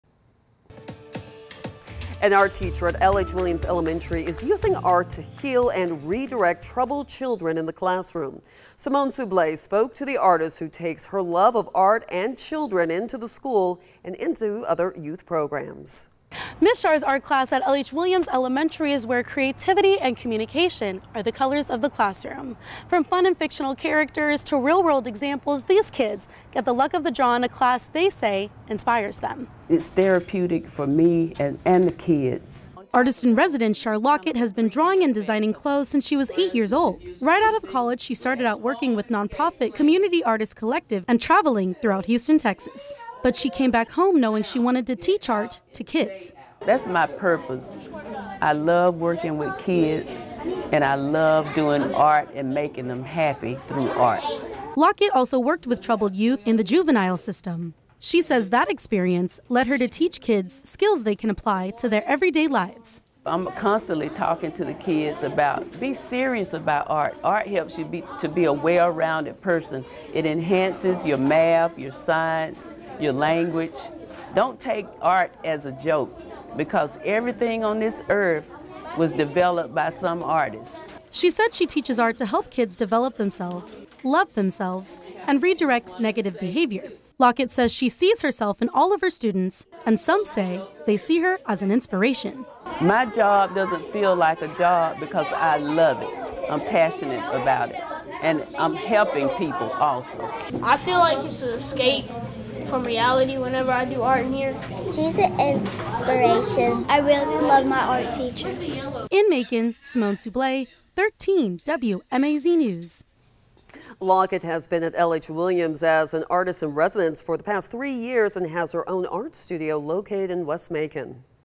Bibb County teacher uses art to help heal students
Lockett teaches art to help kids develop themselves, love themselves and redirect negative behavior.